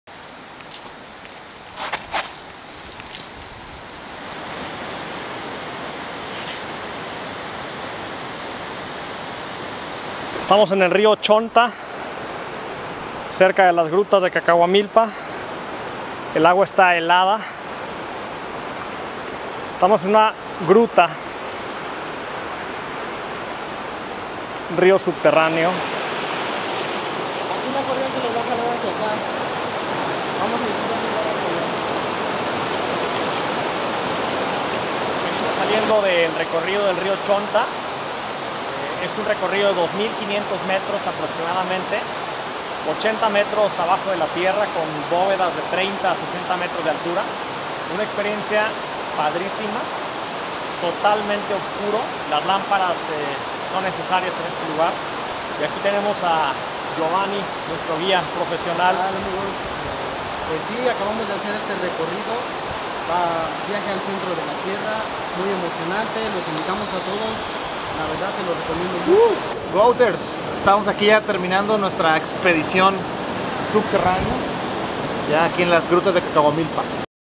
Río Subterráneo Chontalcoatlán 27/mar/2017
¡Una aventura inolvidable! 2.5 km en un río subtarráneo en las Grutas de Cacahuamilpa en Guerrero.